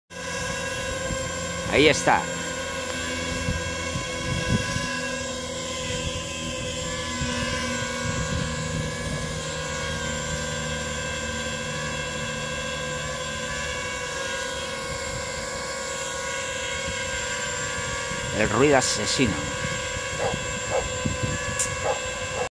ORIO - BASQUE COUNTRY - FPV DRONE KILLER NOISE
This noise produced by an FPV drone in Ukraine and other places at war is called KILLER NOISE. TORMENTA WORK FACTORY DRONES - AGUSTINTXO DRONE PILOT